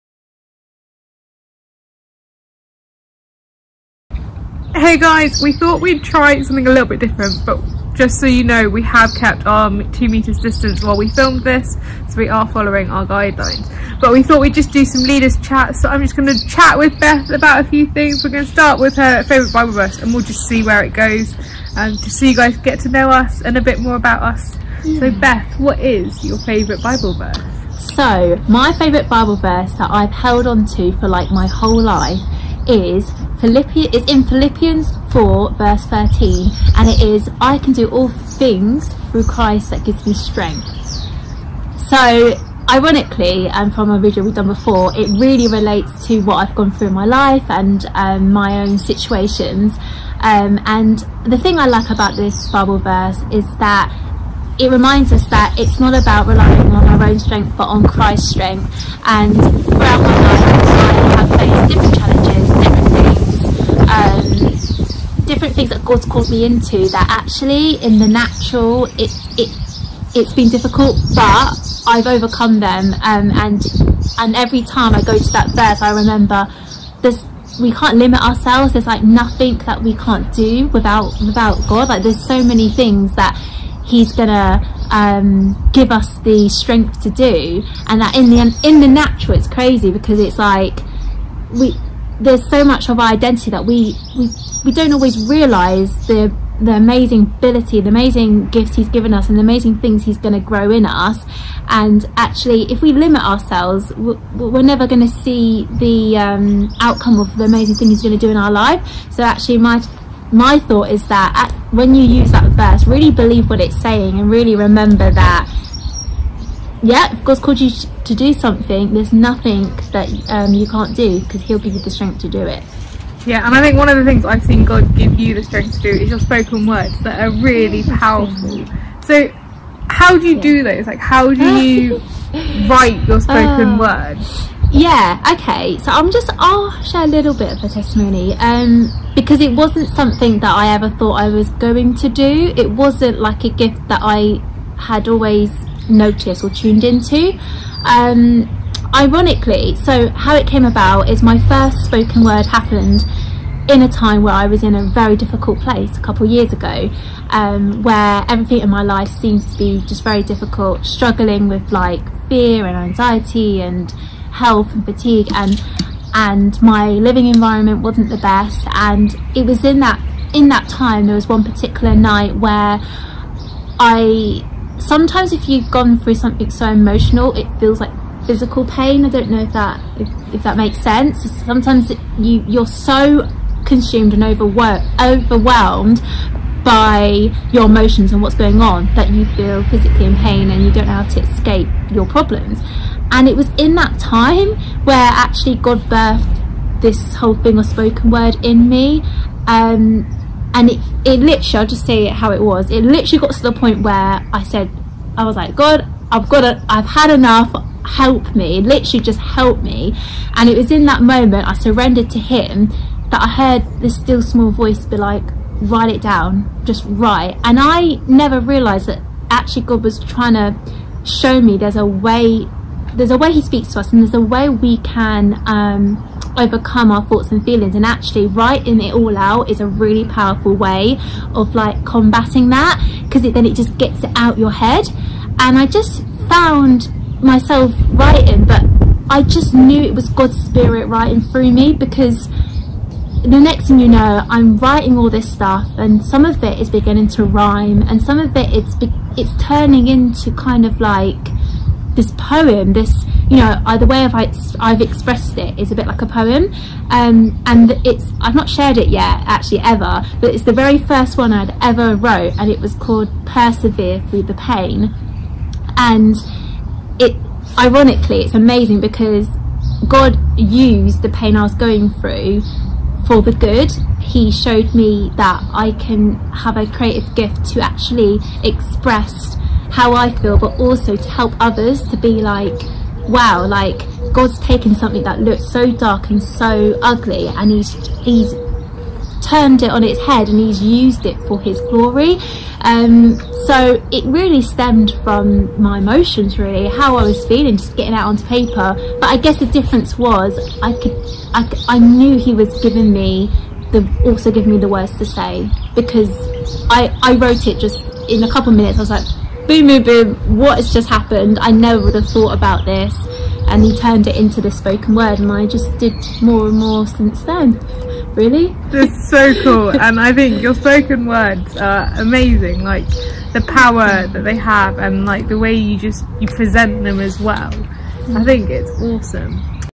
Youth - Leader's Chat with Beth and Sarah (26th June)
In this new style video from our youth team Sarah asks Beth about how she started writing spoken words.